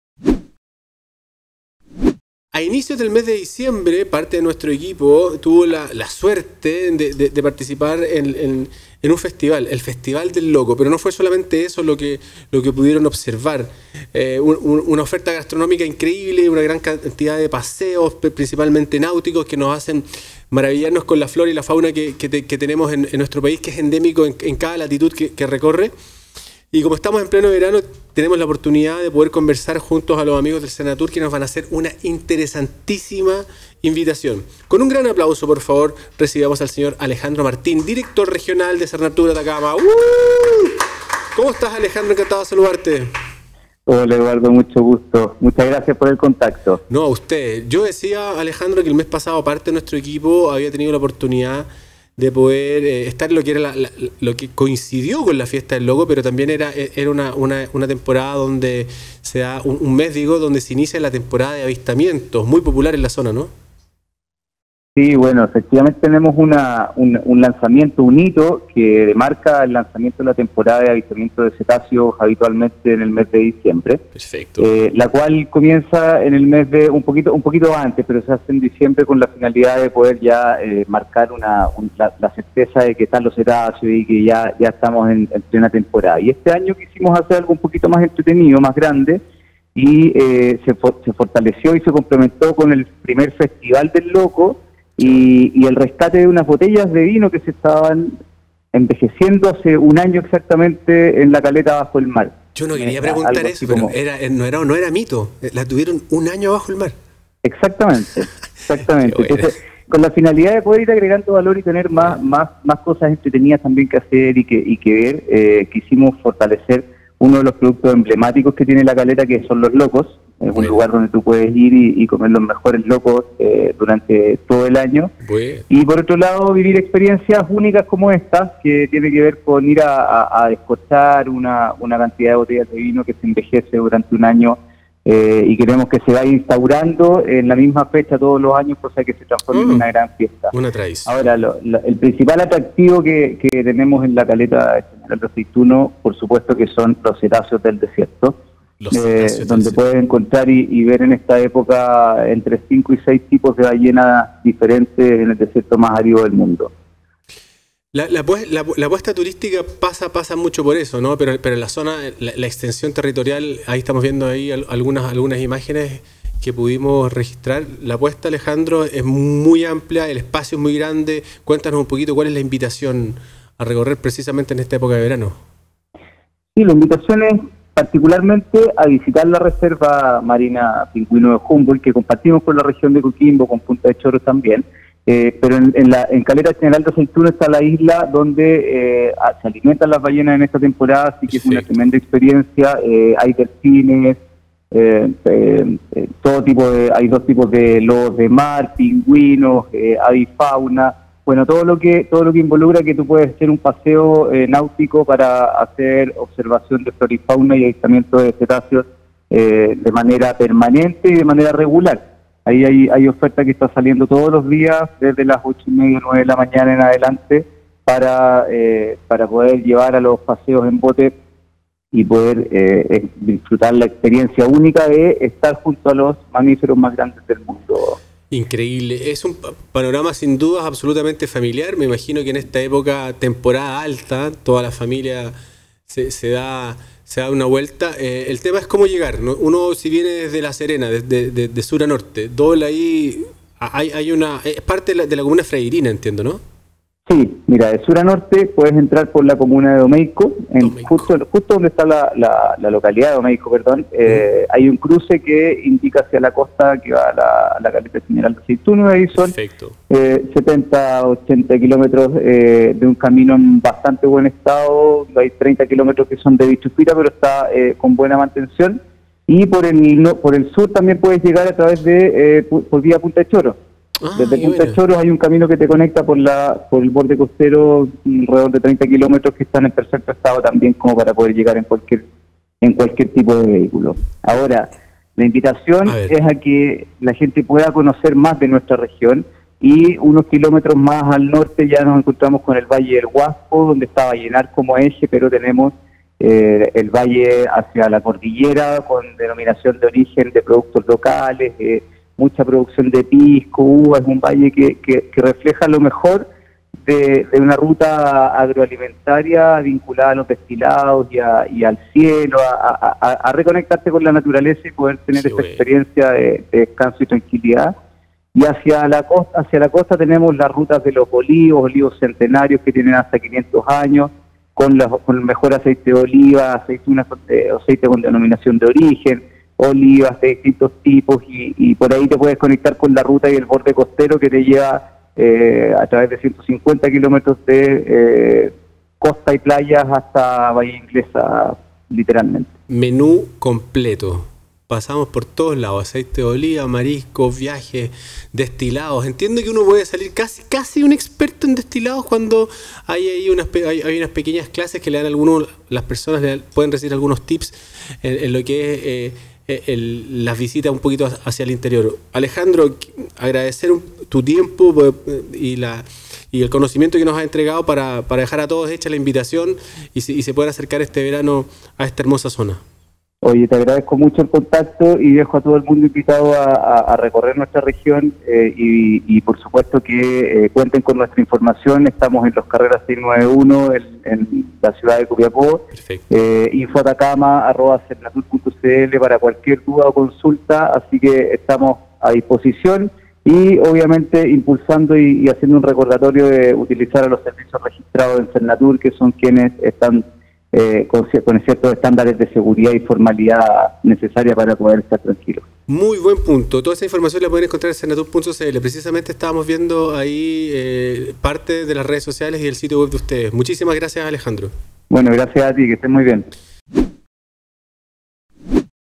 Chañaral de Aceituno🚤
Hoy estuvimos comentando sobre el destino Chañaral de Aceituno, para eso hablamos al Director del Sernatur Atacama.

Esta entrevista fue realizada antes del aluvión que se vivió en los últimos días en la región de Atacama. Como FullOutdoor estamos muy preocupados por lo que está aconteciendo y esperamos que la situación mejore.